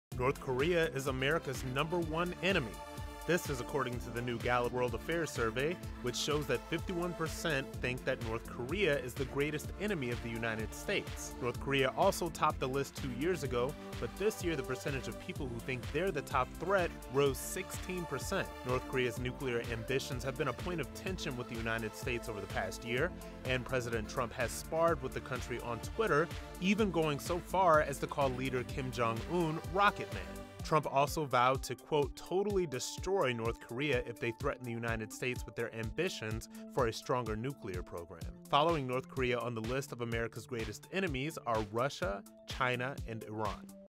Poll: North Korea is America’s biggest threat
North Korea is America’s #1 enemy! Rob Smith (@robsmithonline) has all the details.